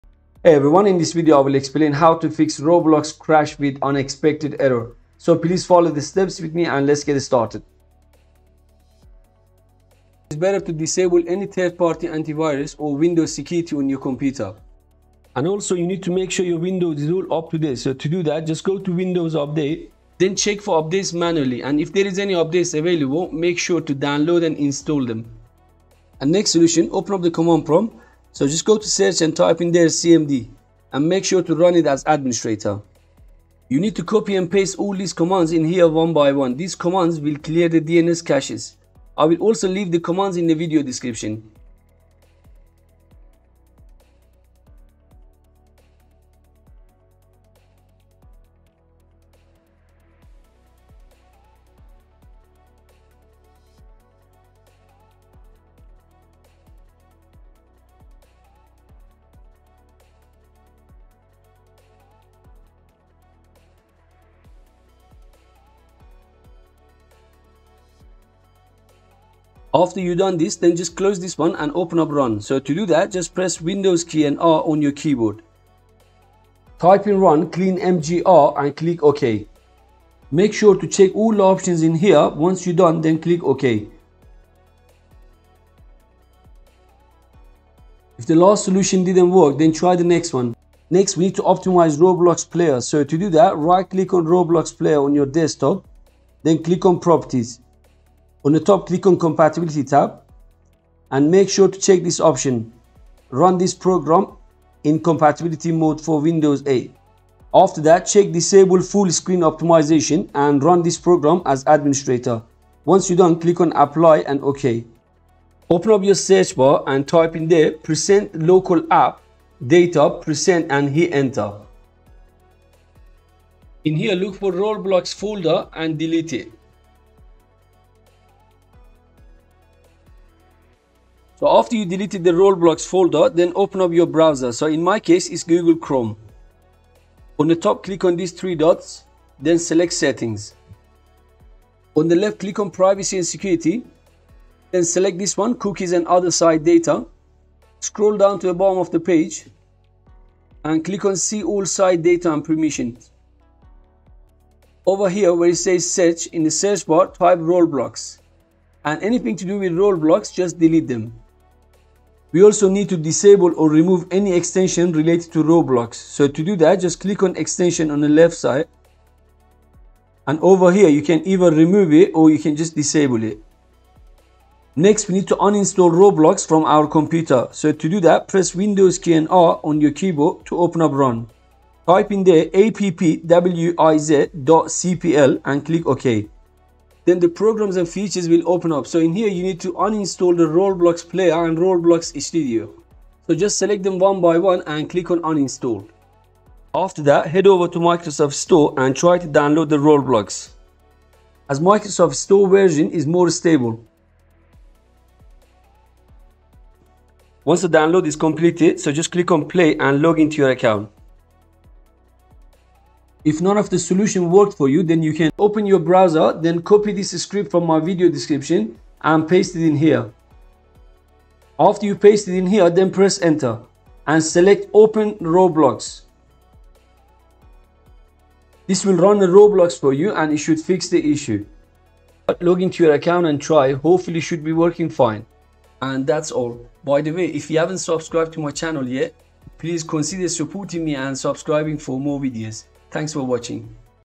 FIXED Roblox Crash AN Unexpected Error Occurred And Roblox Needs To Quit
▶ How To Fix Roblox crash an unexpected error occurred and roblox needs to quit" when trying to play Roblox game on your Windows PC.

In this video I will show you how to fix Roblox crashing issue with the error An unexpected error has occurred and Roblox need to quit. Watch the complete tutorial to fix Roblox crashing issue
Watch full tutorial to fix krnl issue with roblox game.

How to fix roblox an unexpected error occurred and roblox needs to quit"
How to fix no roblox crashing error

CMD Commands:
ipconfig /flushdns
netsh int ipv reset
netsh int ipv6 reset
ipconfig /registerdns

Command to paste in browser:
roblox-player:1+launchmode:app+channel:zwinplayer64